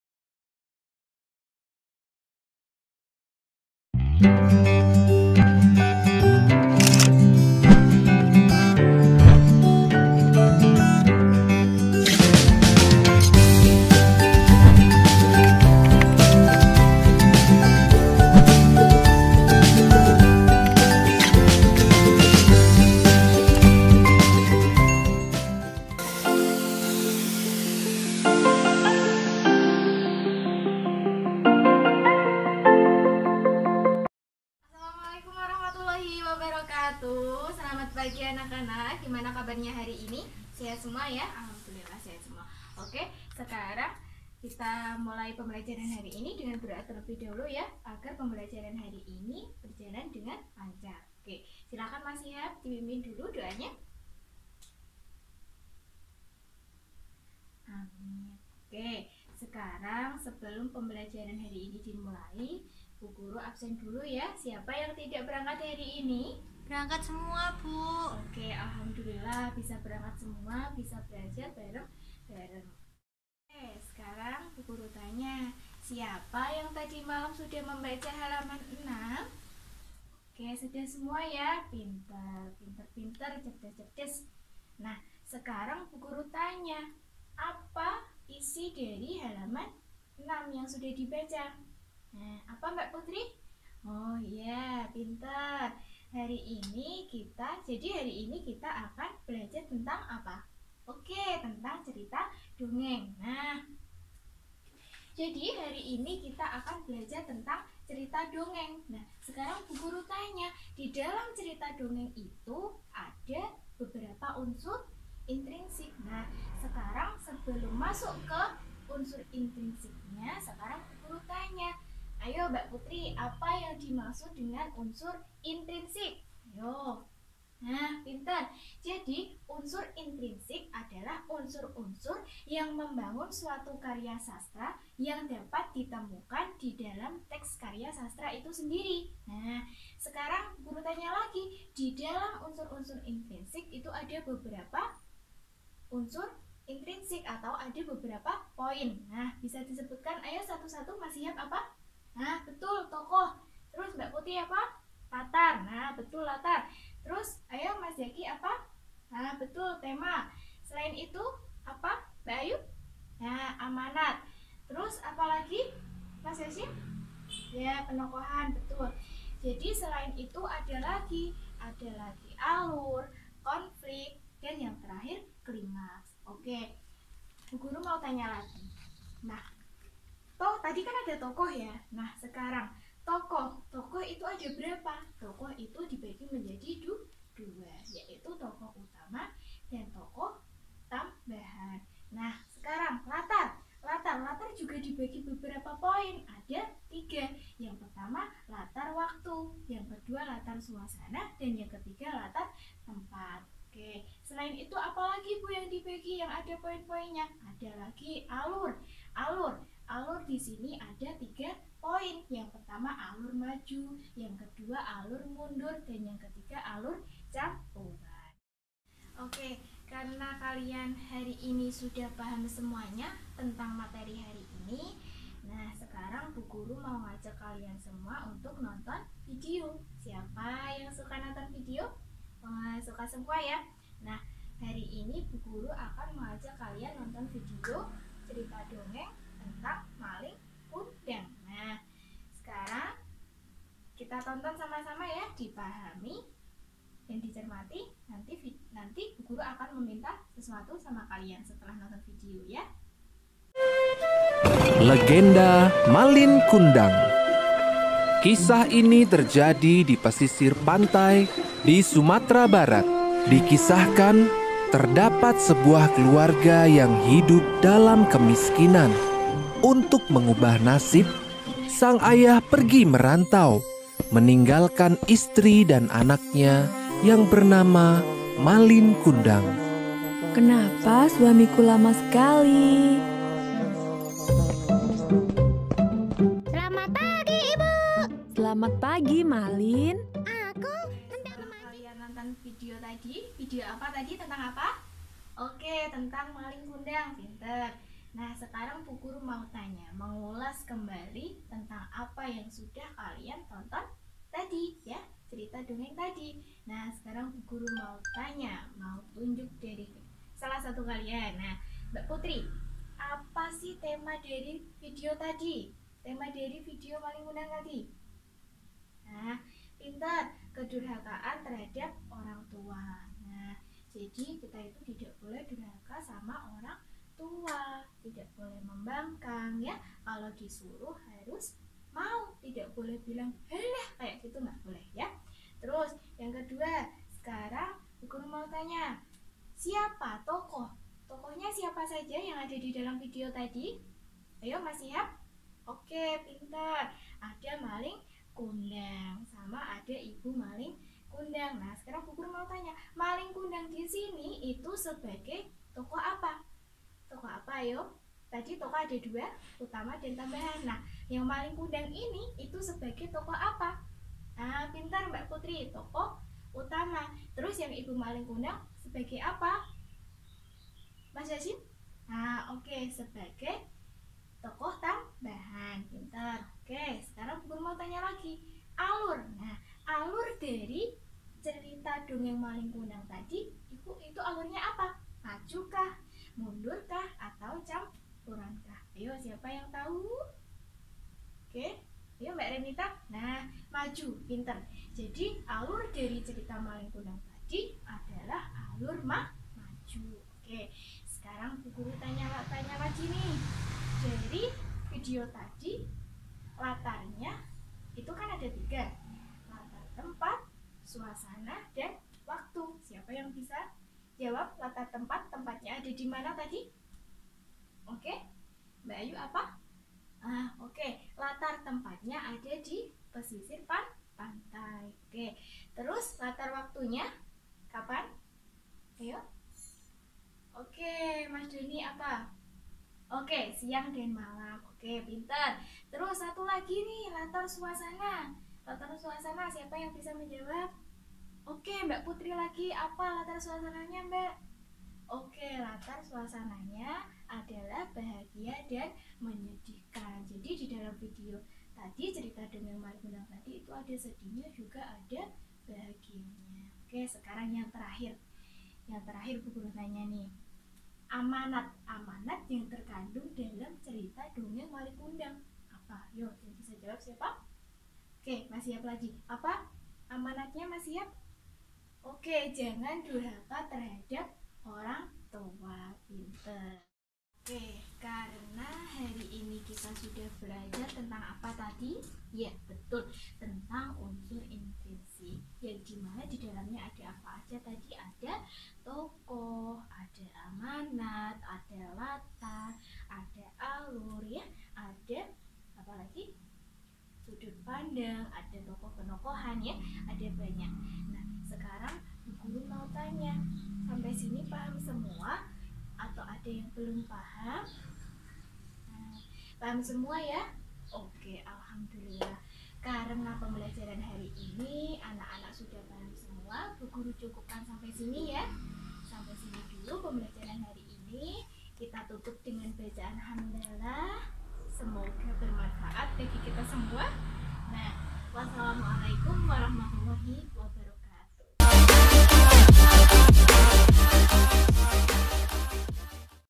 Microteaching B.Indonesia Kelas 4 Tema 4 Sub Tema 1
Gimana kabarnyaa nih .. Baik yaa pastinyaa 😂
Tetap semangat skuyy, mengabdi dari rumah tak jadi masalah 😂

Oke, kak ayu ini merupakan salah satu tim KKN-IK DR IAIN KUDUS Posko 44,
Nah, pada kesempatan kali ini kak ayu akan mempersembahkan microteaching
Kelas : 4
Semester: 1
Tema : 4
Materi : Unsur- Unsur Intrinsik Dalam Cerita Dongeng

Microteaching kali ini memang berbeda,  tetapi kak ayu tetap memberikan yang terbaik dan tetap selalu semangat dong pastinya 😊

Kami sangat membutuhkan kritik dan saran dari kalian semua, jadi kalian bisa memberikan kritik dan saran ke email kami